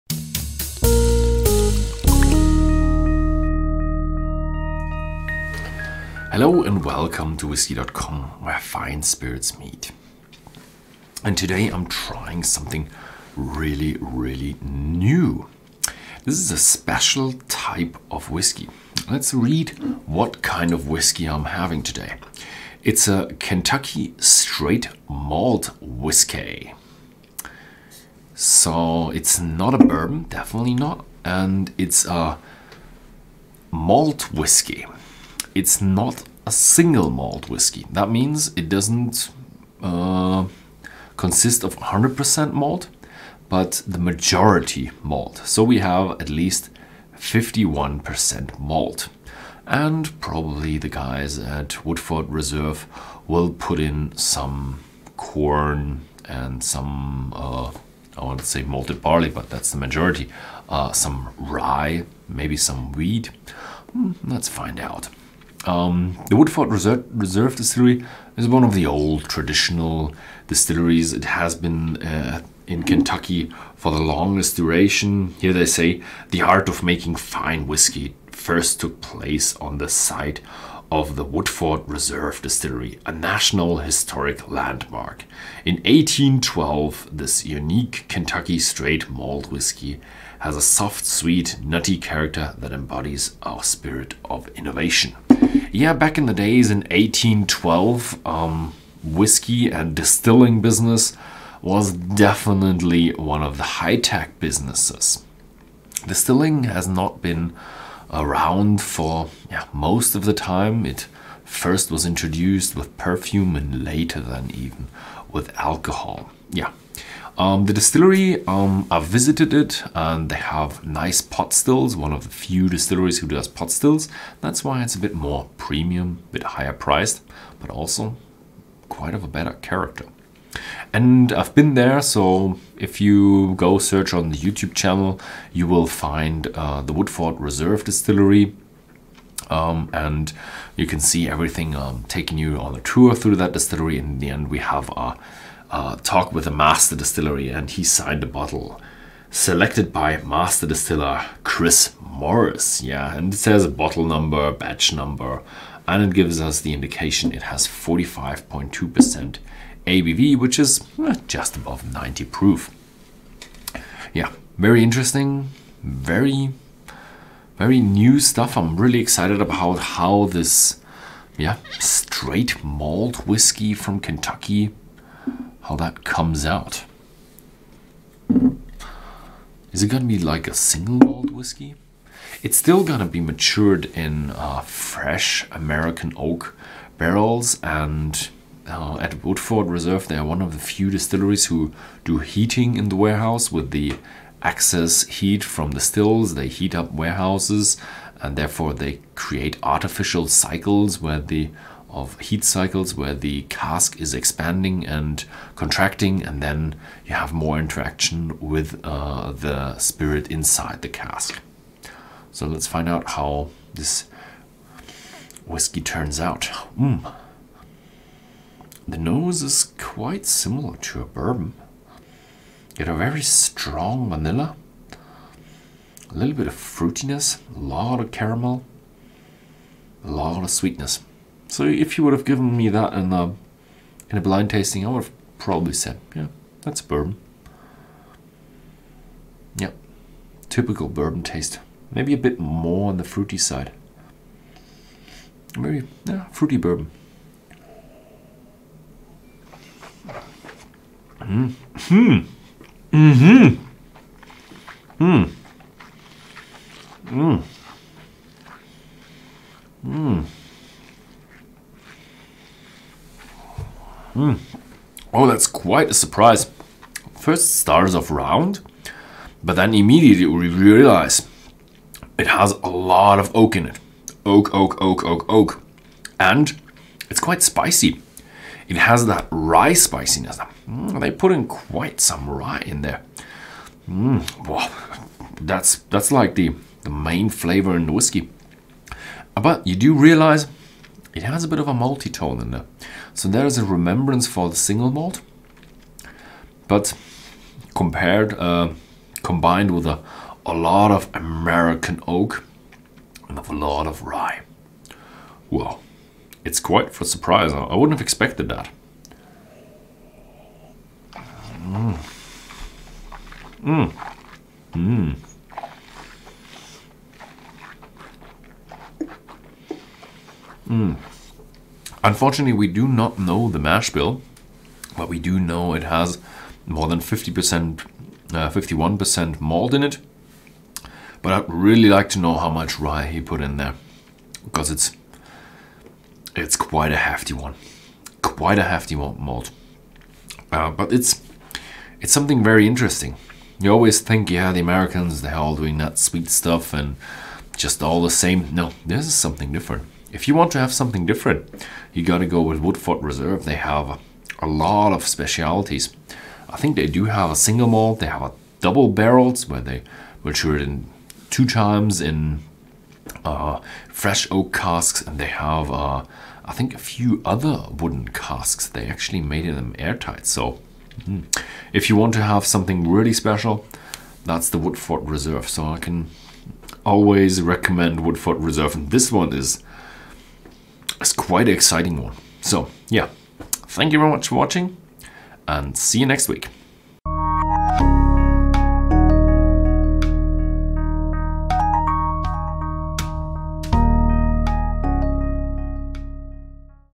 Woodford Reserve Malt
Nosing 2:53
Whisky.com reviews the Woodford Reserve Malt.